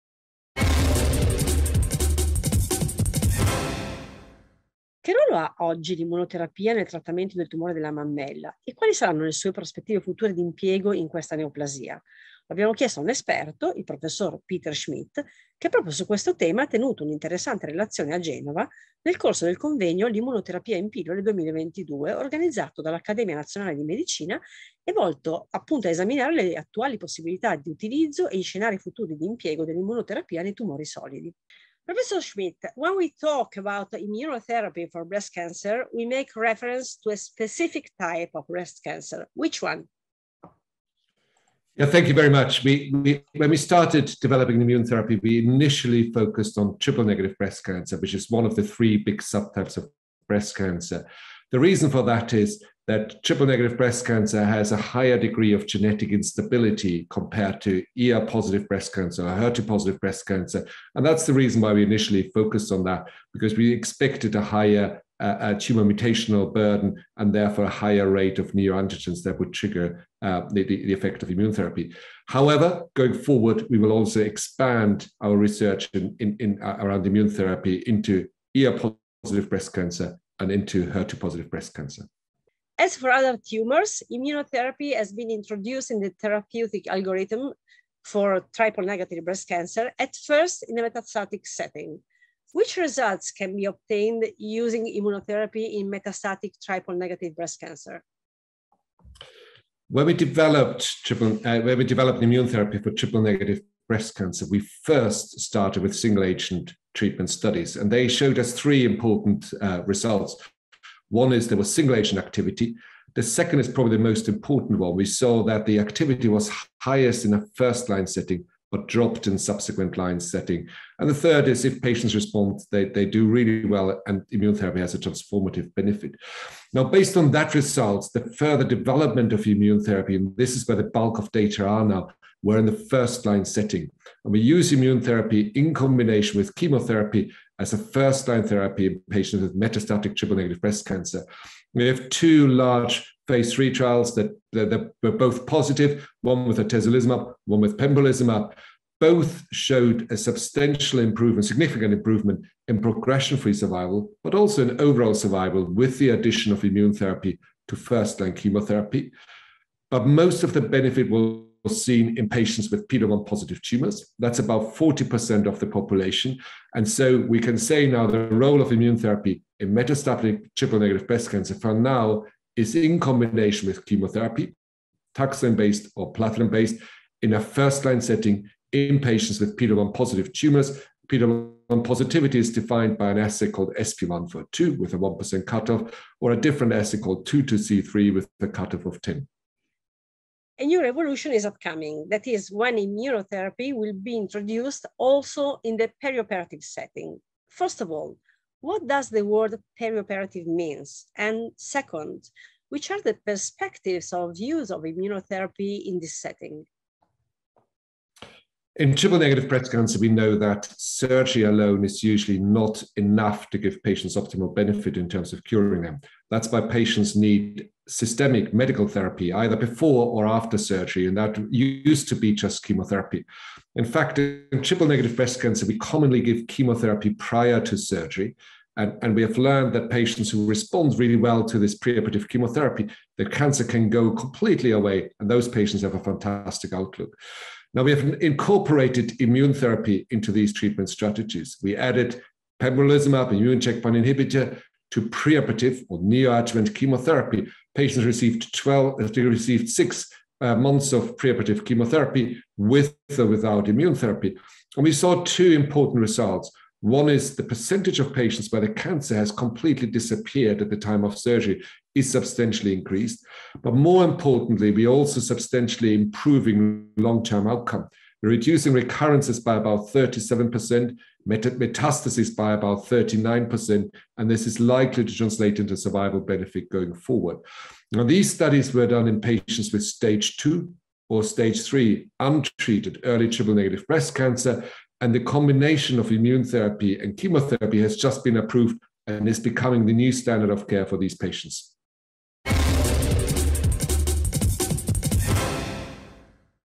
Tumore al seno triplo negativo, ruolo attuale e prospettive future dell'immunoterapia
Che ruolo ha oggi l’immunoterapia nel trattamento del tumore della mammella e quali saranno le sue prospettive future di impiego in questa neoplasia? Lo abbiamo chiesto a un esperto, il Professor Peter Schmid, Direttore del Barts Breast Cancer Centre di Londra, che proprio su questo tema ha tenuto un’interessante relazione a Genova, nel corso del convegno ‘L’immunoterapia in pillole 2022’, organizzato dall'Accademia Nazionale di Medicina e volto a esaminare le attuali possibilità di utilizzo e gli scenari futuri di impiego dell'immunoterapia nei tumori solidi.